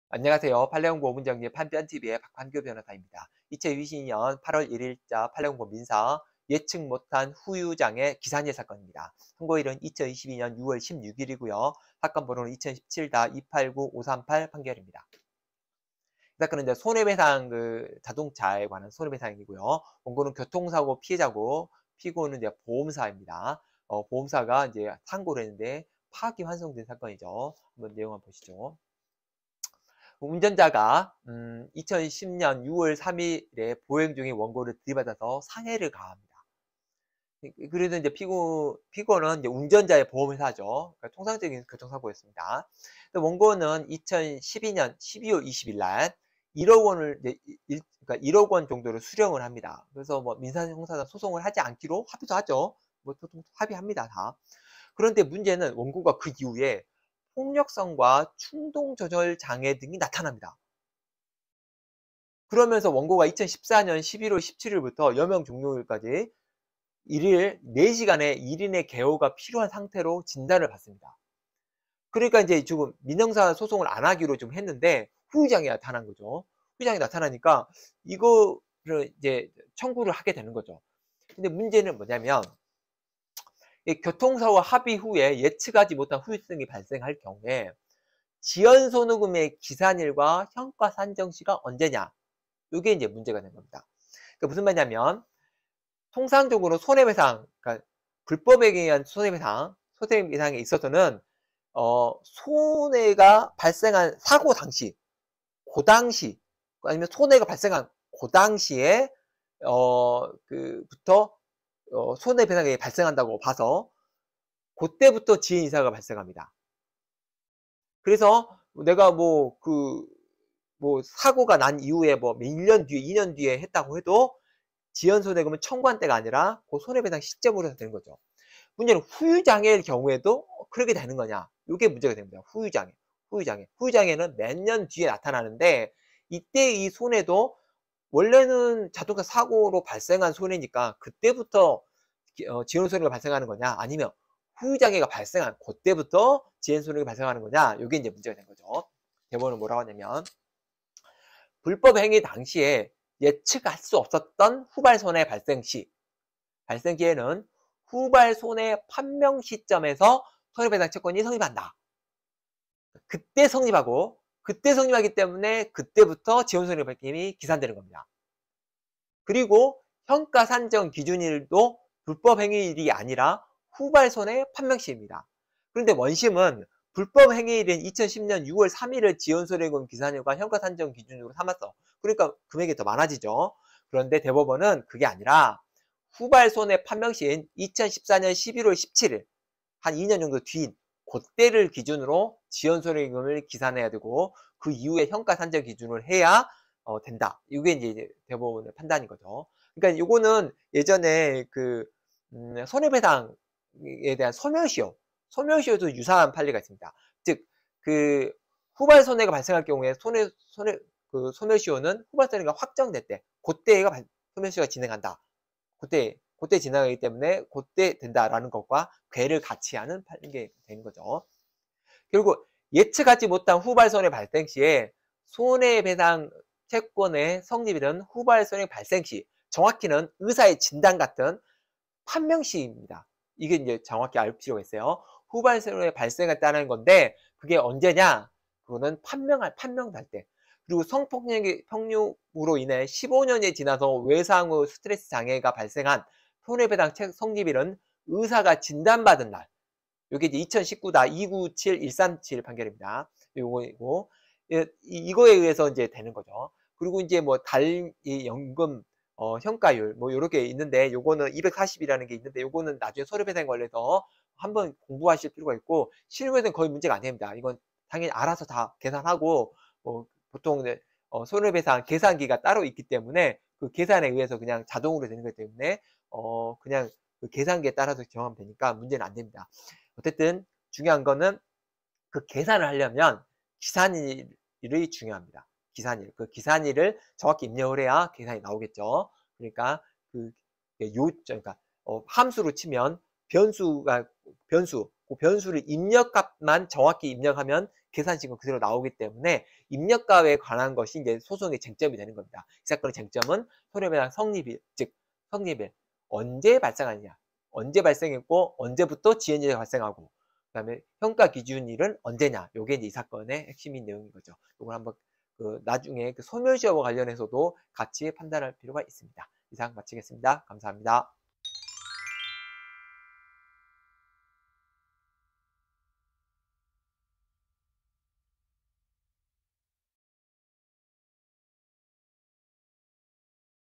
예측 못한 후유장애 기산일 사건 [22.8.1.자 판례공보(민사)]
예측 못한 후유장애 기산일 사건
별 4개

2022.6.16. 선고 2017다289538 판결
손해배상(자)

원고 교통사고 피해자
피고 보험사(상고인)

파기환송

운전자가 10.6.3. 보행중인 원고를 들이받아 상해를 가함
피고는 운전자의 보험회사
원고는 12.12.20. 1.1억원 수령, 민형사상 소송 않기로 합의
원고는 폭력성, 충동조절 장애 등이 나타남
원고는 14.11.17.부터 여명 종료일까지 1일 4시간의 1인 개호 필요

교통사고 합의 후 예측하지 못한 후유증 발생시
지연손해금의 기산일과 현가산정시는 언제?

불법행위 당시 예측할 수 없었던 후발손해 발생시
후발손해 판명시점에서 손배채권 성립, 지연손해금 기산
현가산정 기준시는 불법행위일이 아닌, 후발손해 판명시임
원심은 불법행위일인 10.6.3을 지연손해금 기산일과, 현가산정기준일로 함
대법원은 후발손해 판명시인 14.11.17.을 지연손해금 기산일과, 그 이후를 현가산정기준일로 함

실무활용
예측하지 못한 후발손해 발생시
손해배상채권의 성립일은 후발손해 발생시(정확히는 판명시임)
성폭력 후 15년 지나 발생한 외상 후 스트레스 장애의 손배채권 성립일은 의사 진단받은 날임
단리연금현가율 240은 따로 숙지할 것, 실무에서는 거의 문제 안 됨